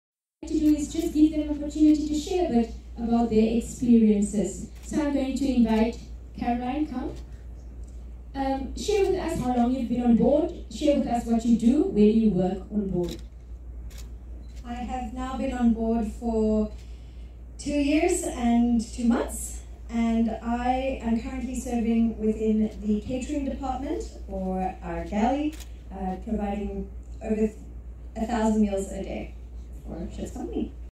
LOGOS HOPE CREW
LOGOS HOPE CREW SHARING THEIR EXPERIENCE WITH THE GUESTS